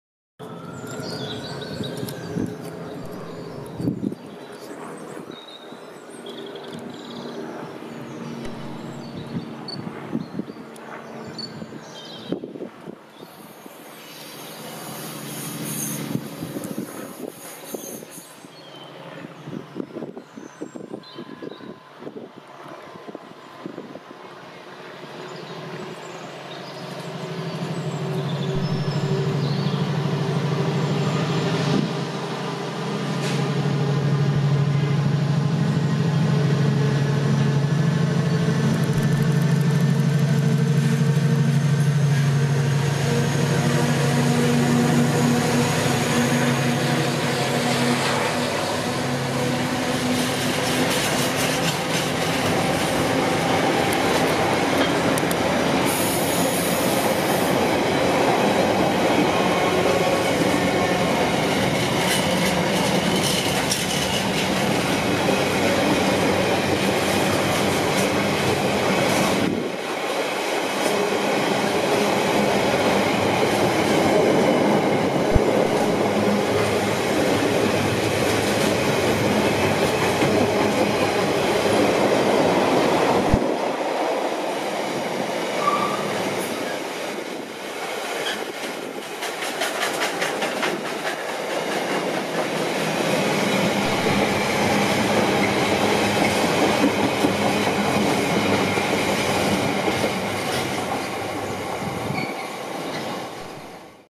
Freightliner 70002 passing Reading West 23rd February 2019
Freightliner class 70 70002 passing Reading West.